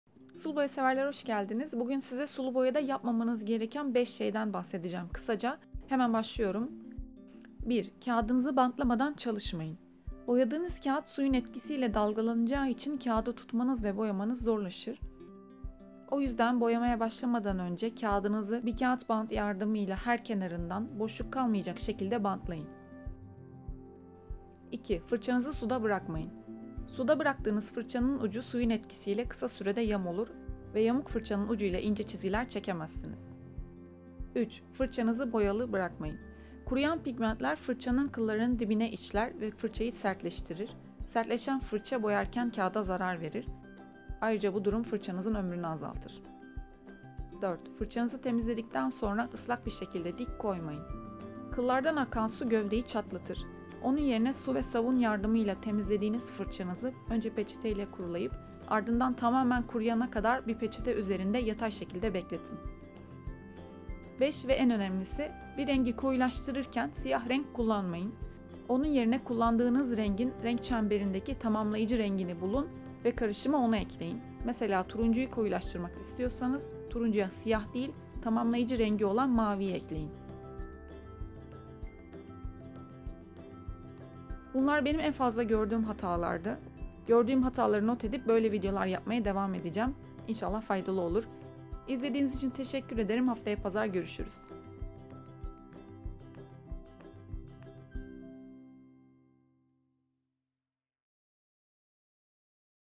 Suluboyada Yapmamanız Gereken 5 Şey
Önemsiz görünen şeyler suluboya performansınızı çokça etkiliyor. Videoda anlattıklarımı uygulamanızı şiddetle tavsiye ediyorum. Bu serinin devamı gelecek.
Bu video tarzını beğendiniz mi? Yorumlarınızı bekliyorum.
Merhaba, ben Müberra. Burada her Pazar saat 18:00'da suluboya çalışmaları ve suluboya hakkında bildiklerimi paylaşıyorum. Bu kanalın iki amacı var:
1- Öğretmek
2- İlham vermek